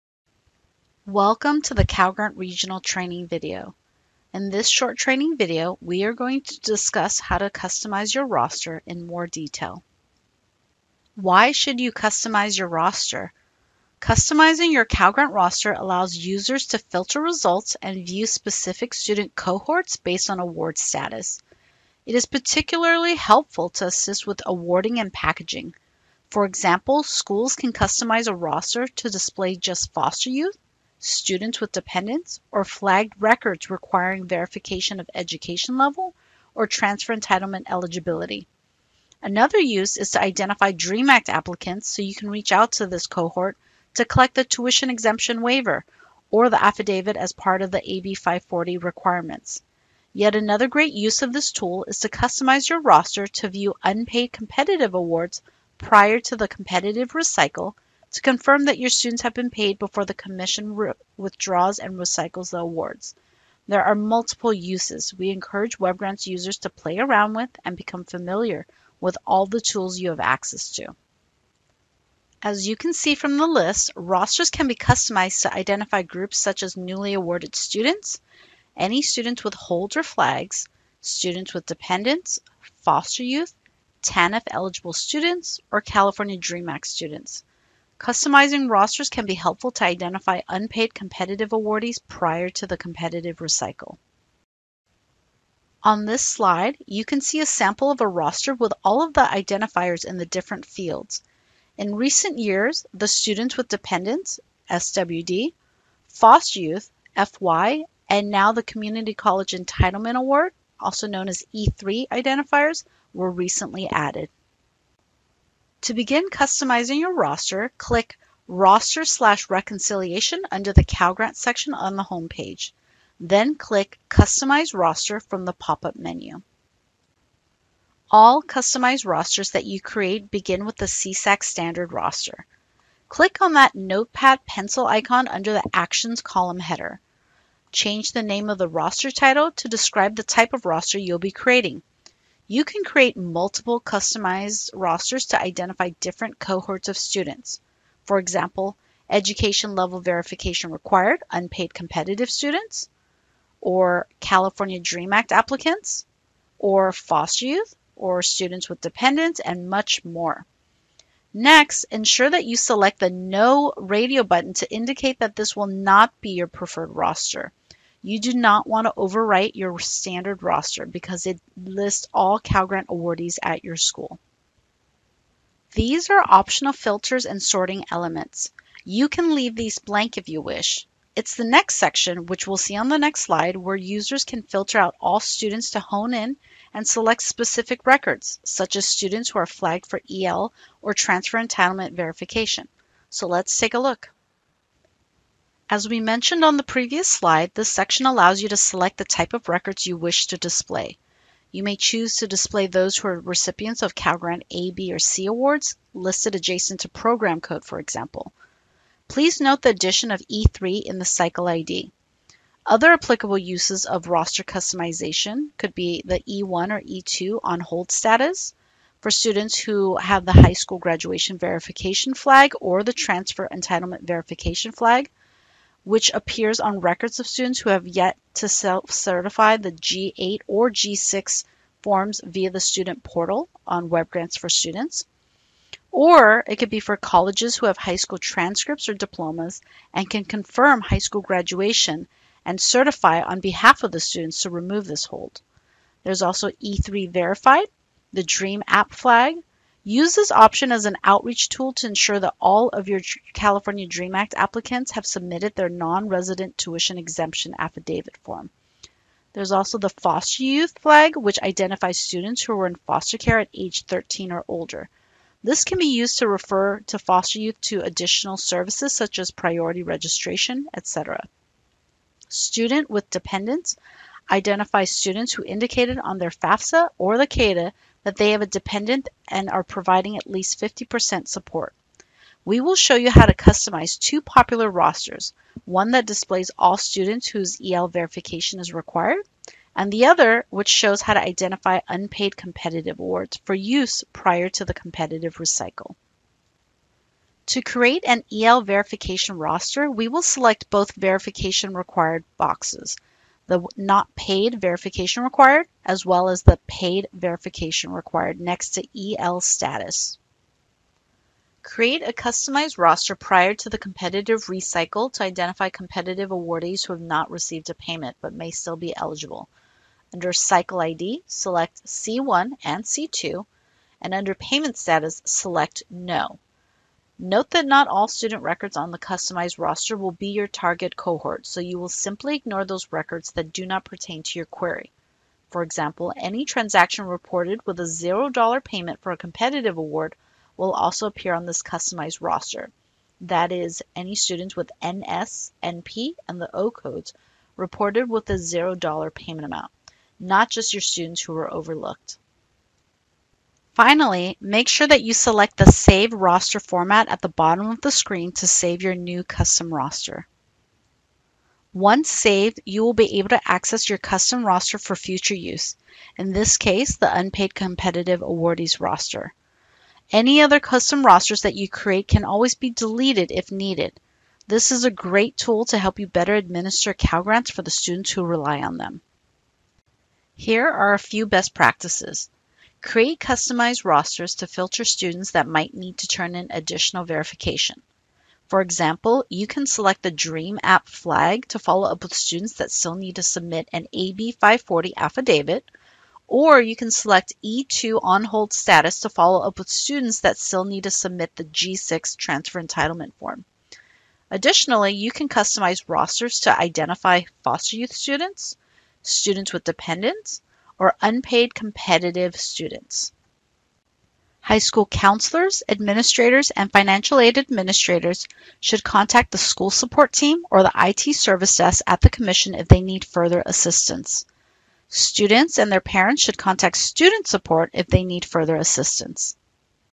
Customizing Roster
In this short training video, we are going to discuss how to customize your roster in more detail.

0:10-1:10 Why customize your roster?
1:11-1:34 Customizable Fields
1:35-1:54 Display Roster
1:55-4:57 Customizing Your Roster
4:58-5:09 EL Verification
5:10-5:52 Unpaid Competitive Awards
5:53-6:00 Save Roster Format
6:01-6:19 Accessing Your Saved Rosters
6:20-6:52 Best Practices